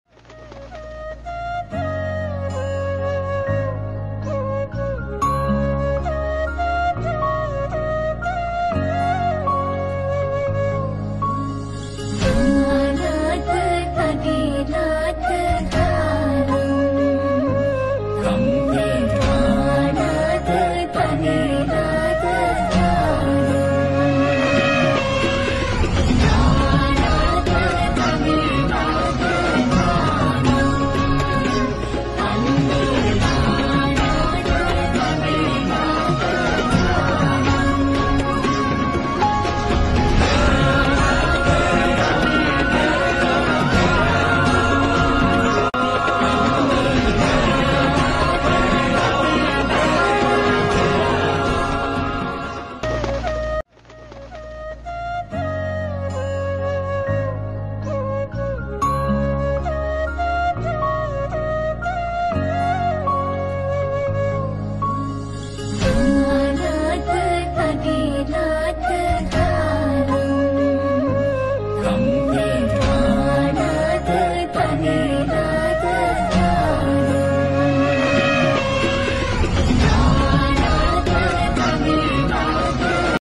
උපුල්වන් දෙවිදු වැඩමවීමට සැරසෙන අවාරයේ කුමාරයා " මිගාර " | Royal Tusker " Migara " | Kelaniya
කැලණිය රජමහා විහාරයේ දුරුතු මහා මංගල්‍යයේ අවසන් මහා රන්දෝලි පෙරහරේ වීබීශන දේවාලයේ දේවාබරන වැඩමවූ ශ්‍රී දළදා මාළිගාවට අයත් " මිගාර " ඇතු ❤️🙏